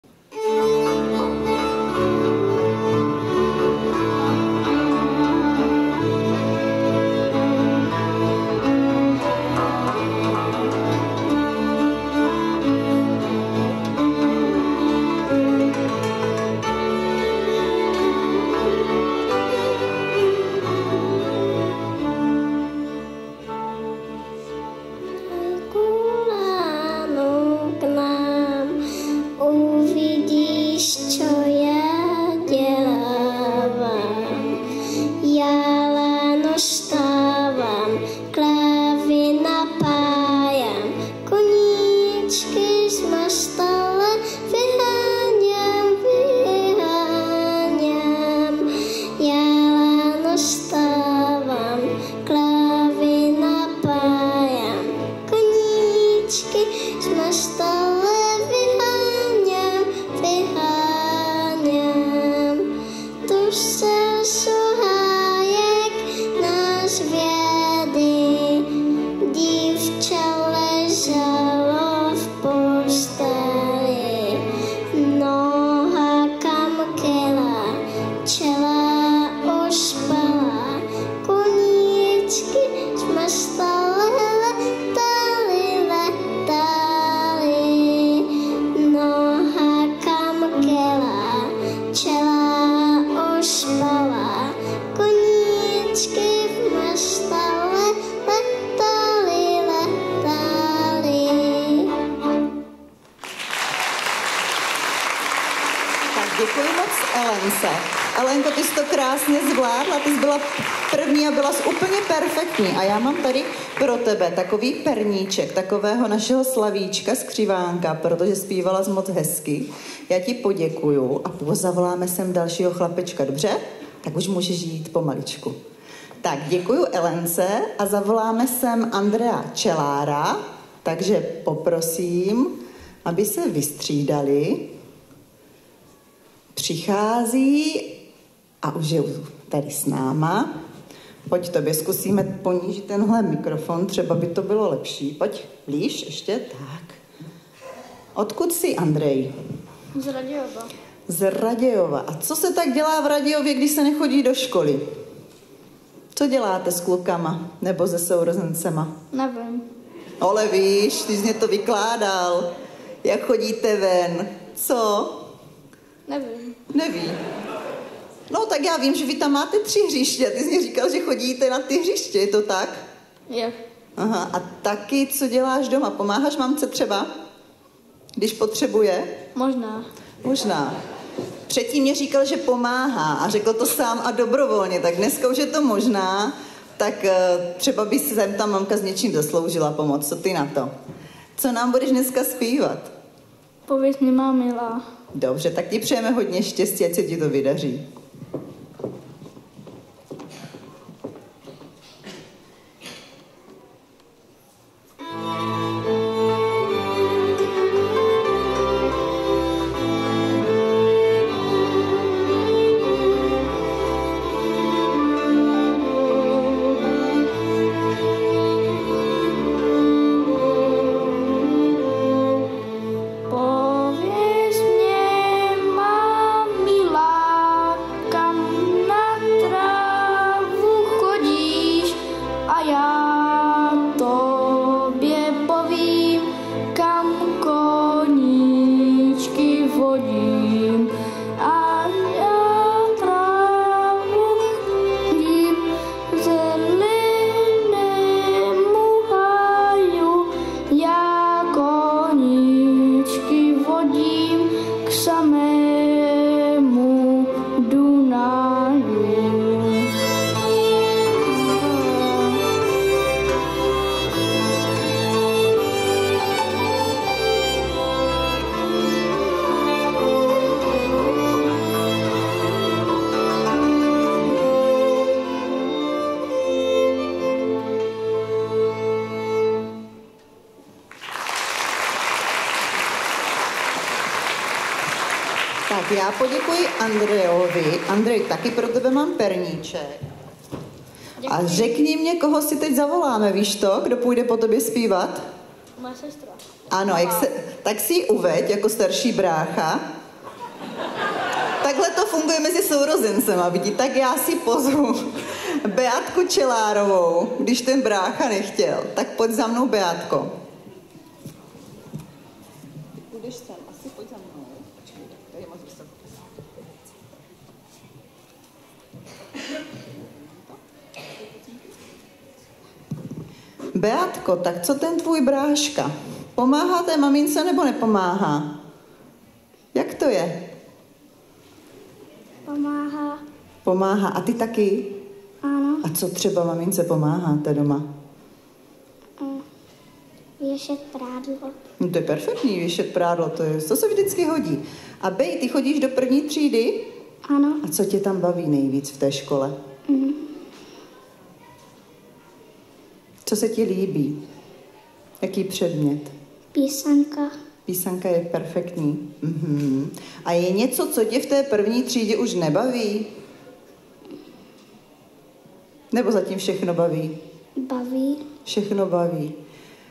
STRÁŽNICE-ZPĚVÁČEK STRÁŽNICKA K POCTĚ VAŠKA HARNOŠE-kategorie do 10 let,doprovod CM J.ŠŤASTNÉHO (2.)
- 14. dubna 2024, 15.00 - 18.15 hodin, kulturní dům Strážničan